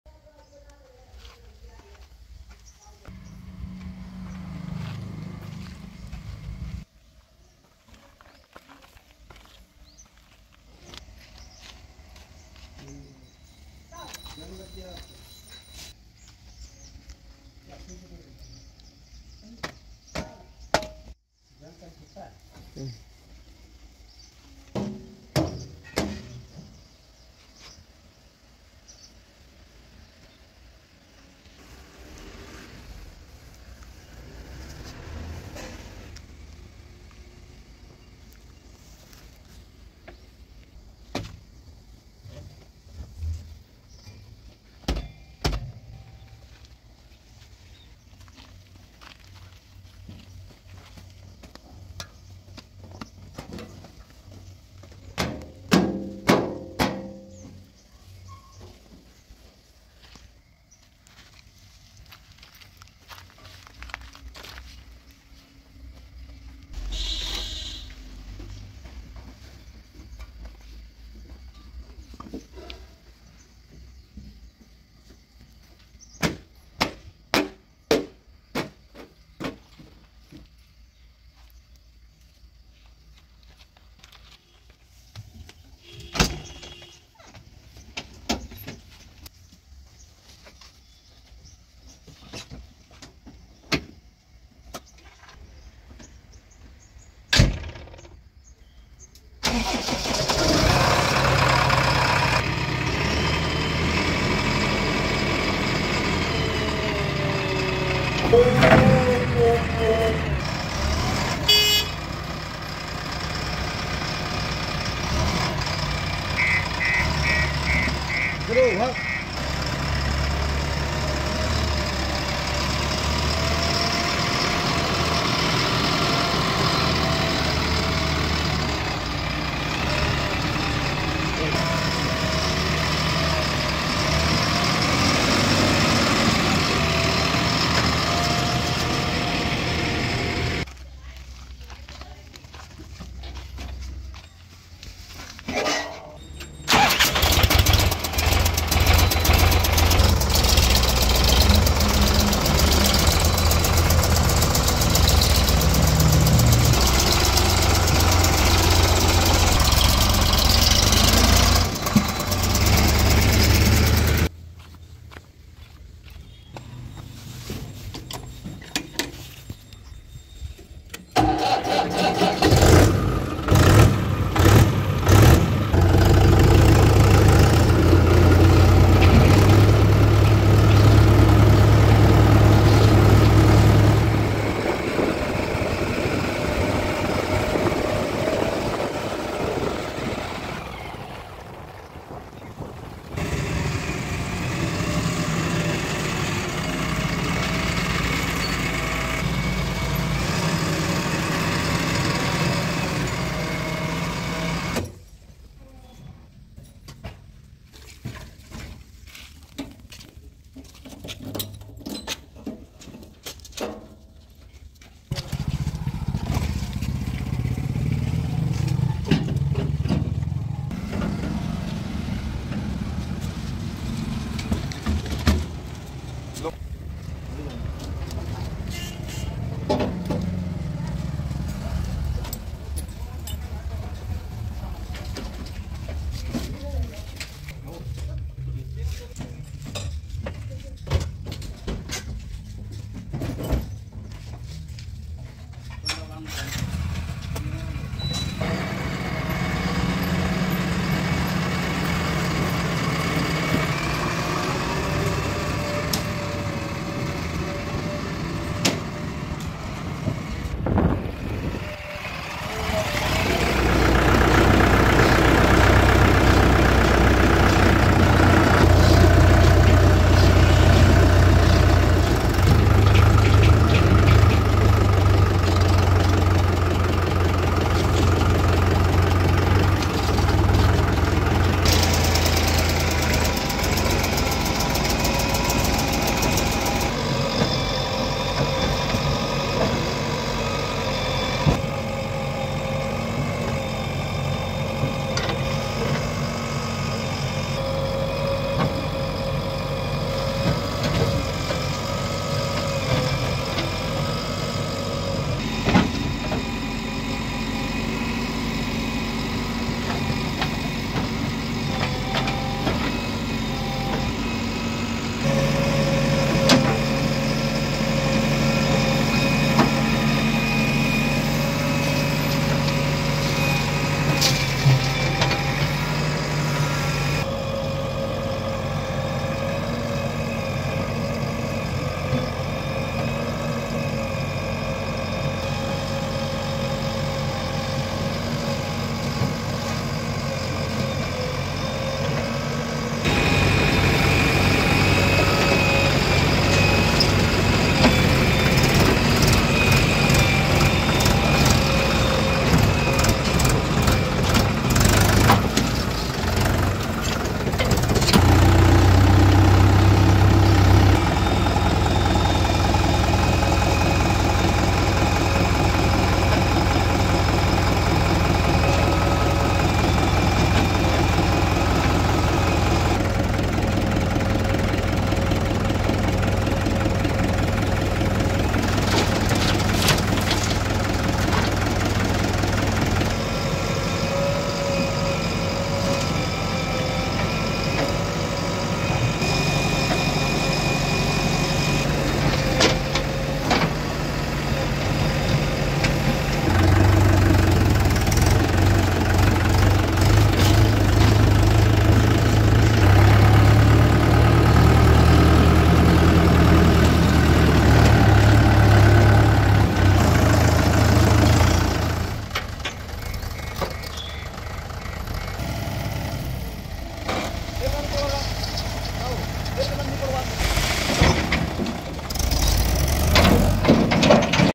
Jcb 3dx Eco Excellence Backhoe Machine Loading Mud In Mahindra and Swaraj Tractor
About Video:In this video a jcb 3dx backhoe loader machine loading mud in Mahindra and Swaraj tractors 😀 🚜

By this video you can watch the performance of jcb machine and both Mahindra and Swaraj tractors power 💪 🚜